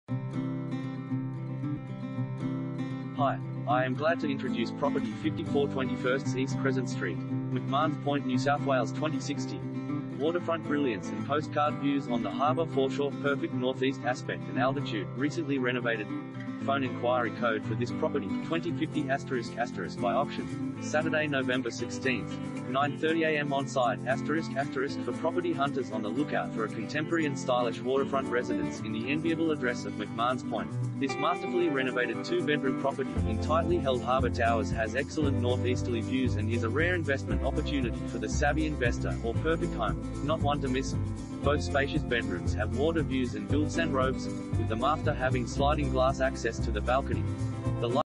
54/21 East Crescent Street, McMahons Point NSW 2060 | AuPropertyReport.Com
54/21 East Crescent Street, McMahons Point NSW 2060
Property Type: unit
Bedrooms: 2
Bathrooms: 2
Car Space: 1
Waterfront Brilliance And Postcard Views On The Harbour Foreshore / Perfect NorthEast Aspect and Altitude / Recently Renovated